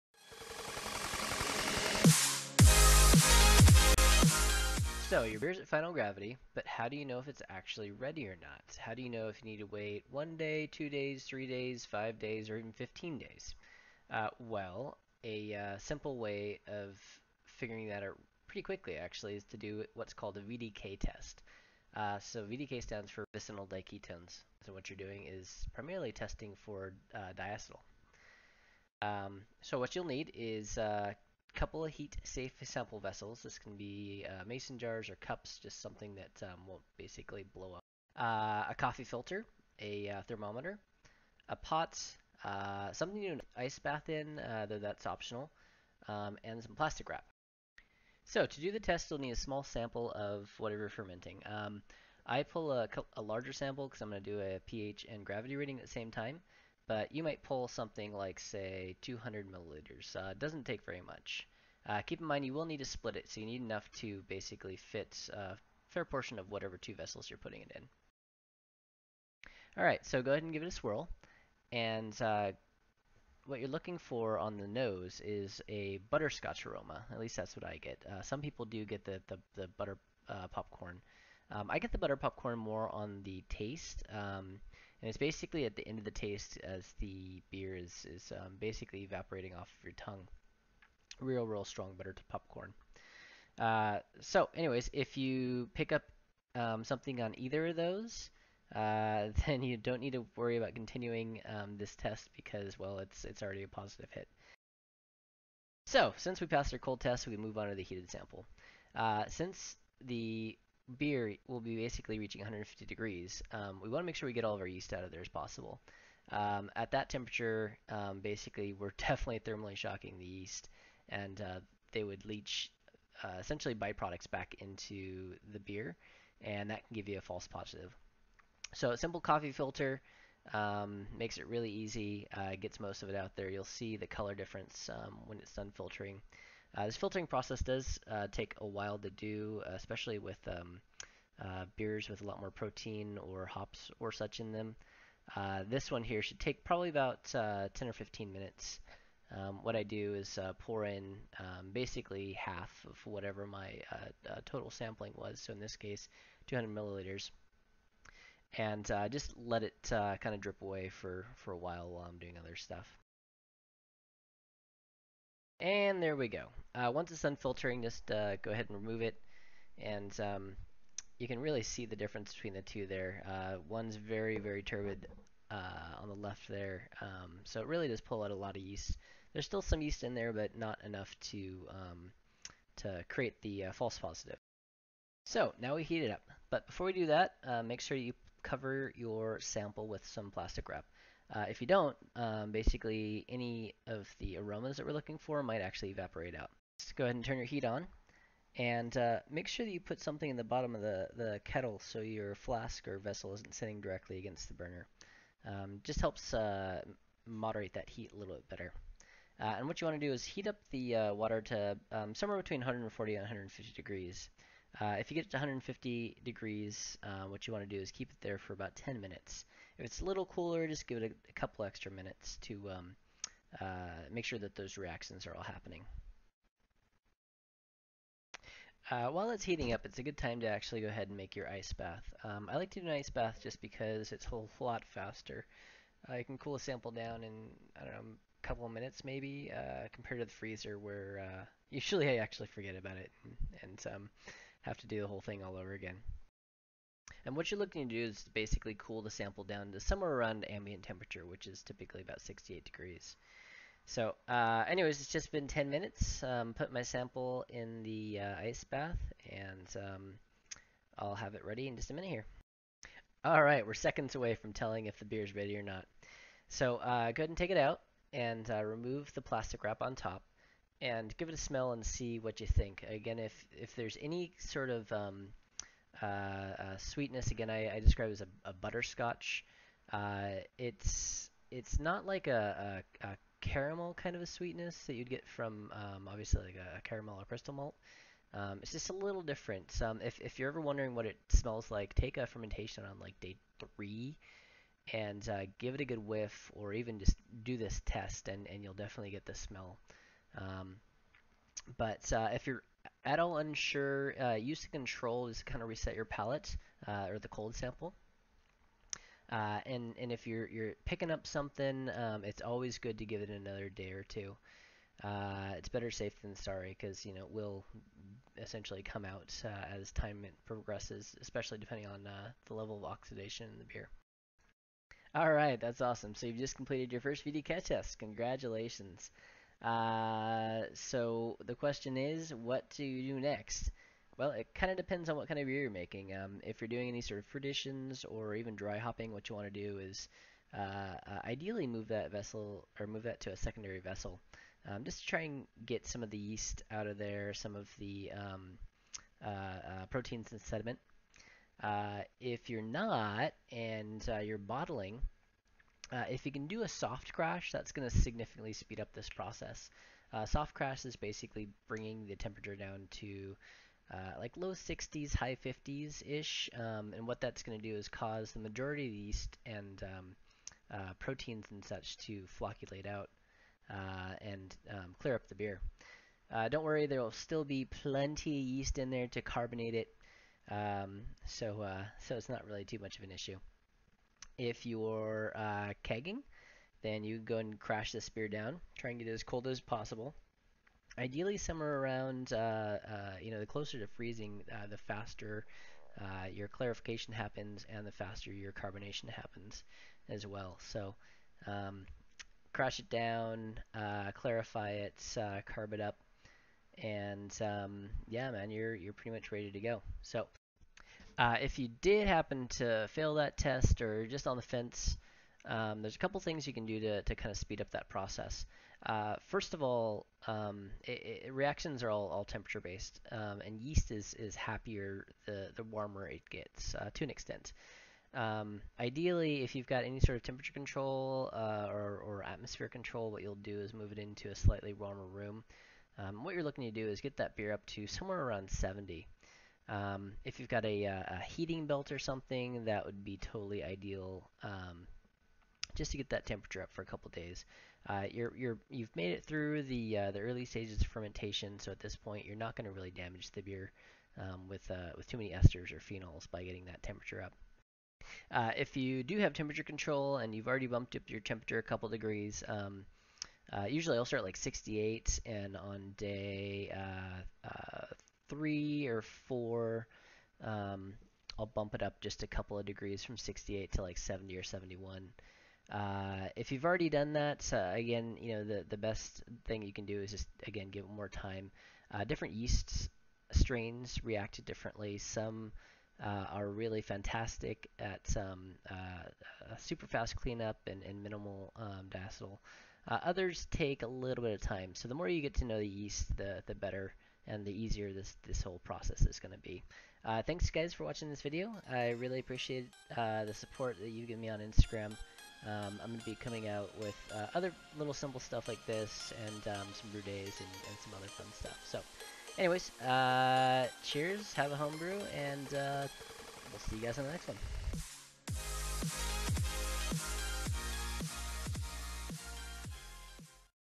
Is it ready yet? How to perform a VDK test to see if your beer is ready after final gravity.
So your beer just hit it's final gravity, but how much longer do you wait until post fermentation clean up is complete? Is it ready now? This test will give you that answer!

VDK Test:
1) Take a minimum 200 ml sample from your fermenter
2) Split it into two heat safe jars
3) Cover with plastic wrap and heat one sample to 150F and hold the temperature for 10 minutes
4) Cool the sample down to 68F, remove the plastic wrap to smell
5) If you smell butterscotch or butter popcorn even slightly, your beer isn't ready
6) Watch video for full details and what to do if your sample fails to pass